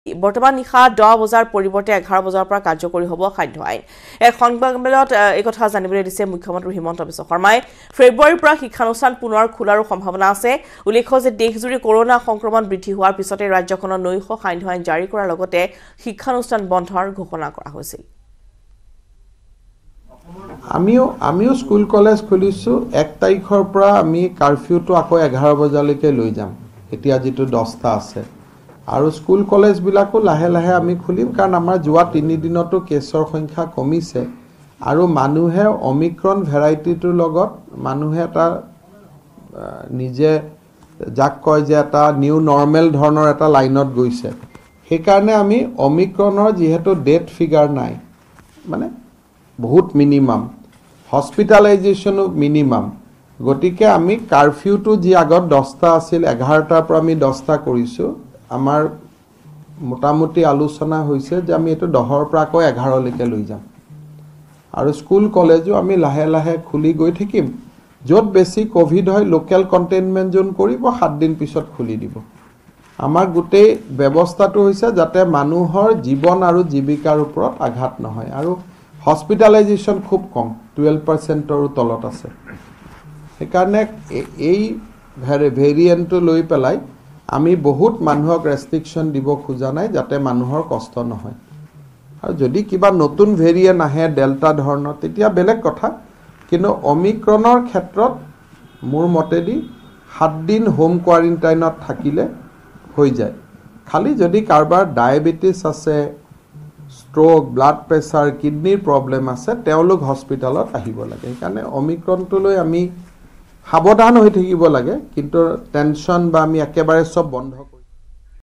Himanta Biswa Sarma | ১০ বজাৰ পৰিৱৰ্তে এতিয়াৰ পৰা ১১ বজাৰ পৰাহে সান্ধ্য আইন
24 January, 2022 : ফেব্ৰুৱাৰীৰ পৰা বিদ্যালয় সমূহ খোলাৰো সম্ভাৱনা।  লগতে চাওঁক আন আন গুৰুত্বপূৰ্ণ সংবাদসমূহ ।